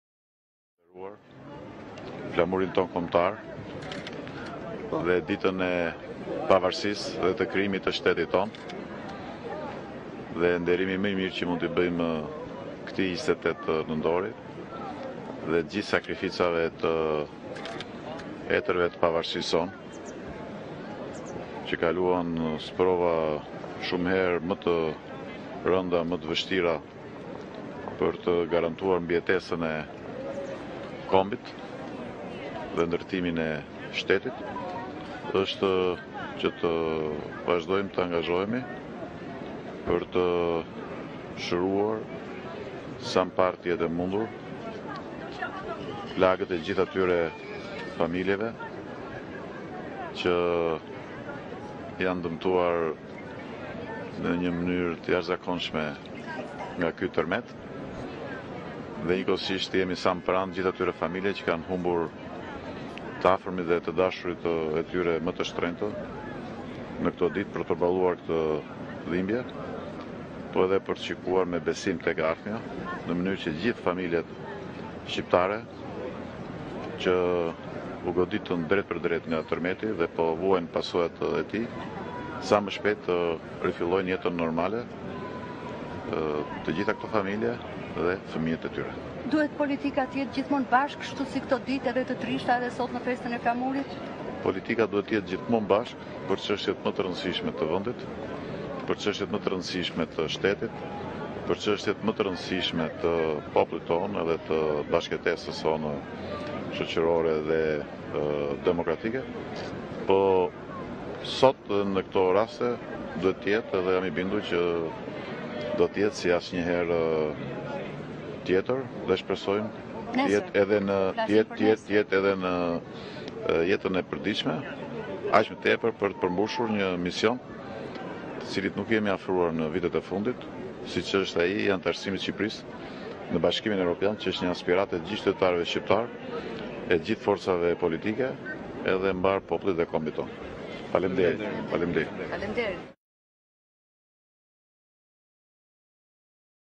Shqipëria 107 vjet e pavarur bën bashkë Presidentin e Kryeministrin në Vlorë, Meta: Nderimi ...
Presidenti Ilir Meta e kryeministri Edi Rama, kanë qenë bashkë në sheshin Pavarësia në Vlorë në kuadër të aktivitete të 107 vjetorit të pavarësisë.

Në një prononcim të shkurtër për mediat, Meta tha se nderimi më i mirë që mund ti bëjmë këtij 28 nëntori është të vijojmë të angazhohemi për të shëruar plagët e gjithë atyre familjeve që janë dëmtuar nga ky tërmeti i 26 nëntorit.